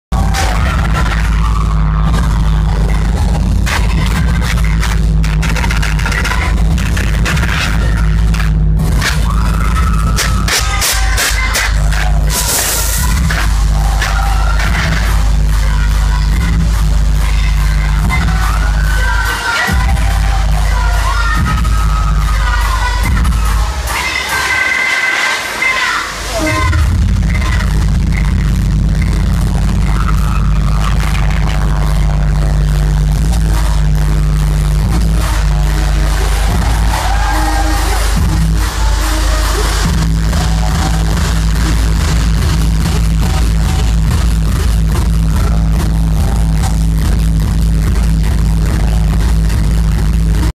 Stage Mounted Co2 Jets and Cryo Gun by CryoFX
CryoFX Stage Mounted Co2 Jets and Cryo Gun going off at a local club.